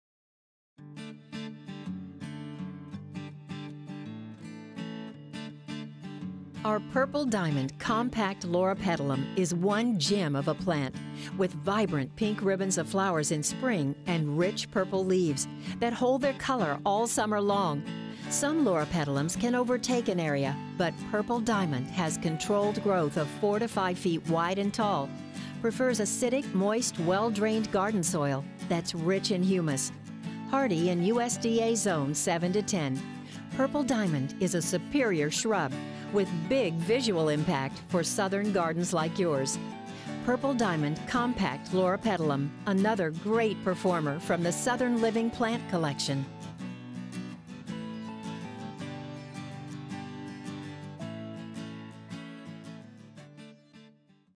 Purple Diamond Compact Loropetalum - A Purple Plant That Won't Grow Too Huge - From Southern Living
Combining colorful flowers and foliage with controlled growth, Purple Diamond is a gem. Unlike some other loropetalums that can swallow your house, this one grows 4 to 5 feet tall and wide. Showy pink flowers appear in spring. Deep, rich purple foliage holds its color all season.